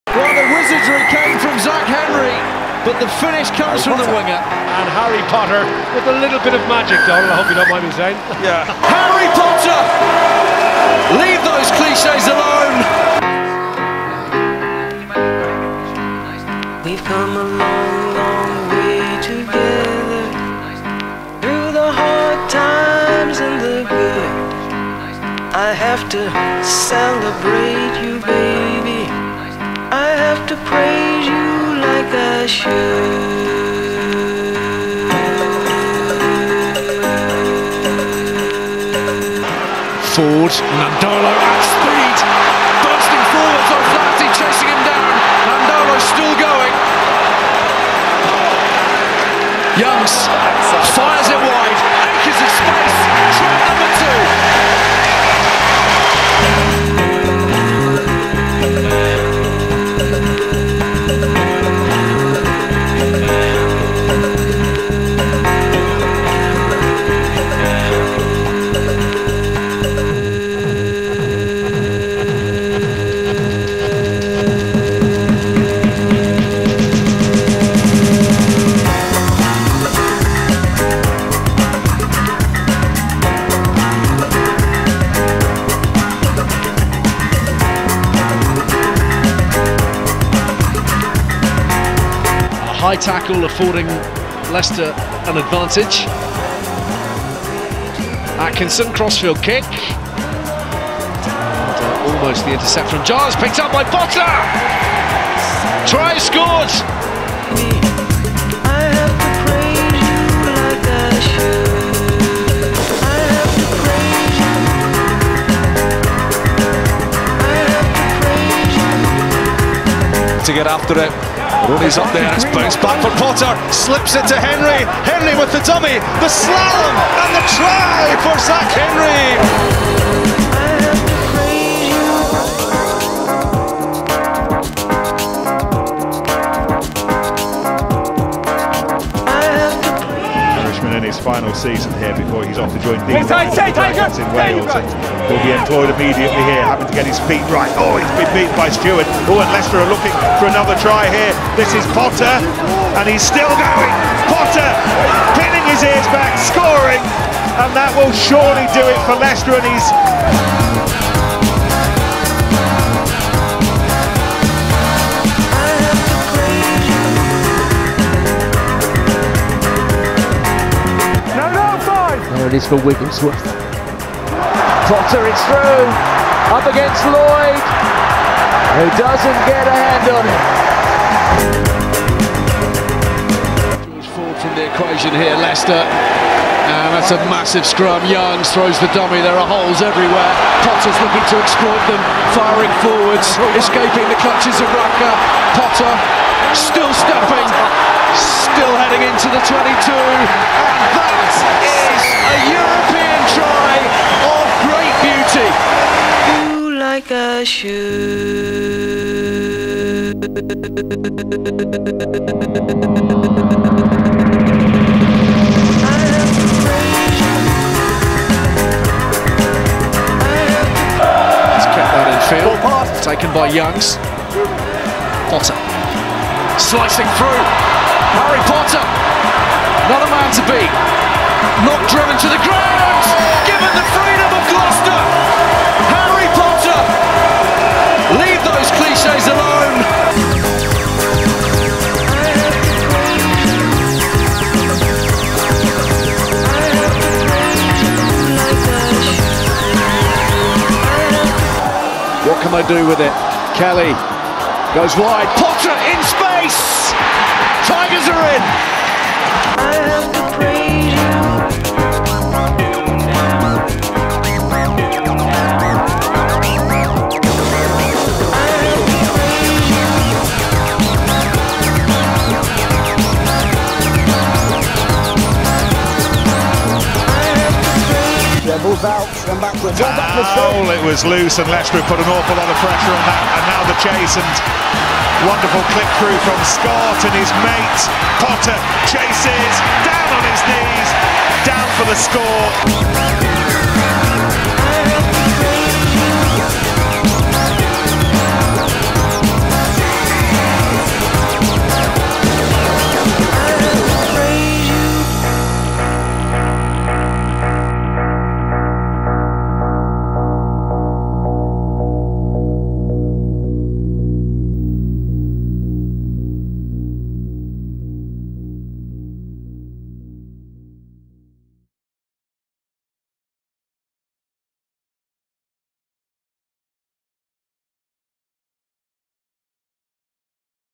Harry Potter - The Magician | Leicester Tigers Leaver's Rugby Tribute
Having recently requested to terminate his contract at Leicester early for an opportunity to move back to Australia and, potentially, play for his adopted country at the World Cup, Harry Potter has left an indelible mark at Welford Road. He will be most fondly remembered for his electric pace, dazzling footwork and extraordinarily slippery approach to evading tackles, but Potter also became a reliable cog in the backfield; perfecting many try-saving tackles.

Though I've covered him before in a previous tribute, Potter has simply played too well over the years to not have an expanded tribute featuring his greatest tries, assists, kicks and defensive displays.

I feel confident in speaking on behalf of the Tigers faithful that Harry Potter will be sorely missed, a player whose absence will be very difficult to fill. This...is his Leaver's Tribute.

Footage:
BT Sport
EPCR
Premiership Rugby

Thumbnail Photo:
Leicester Tigers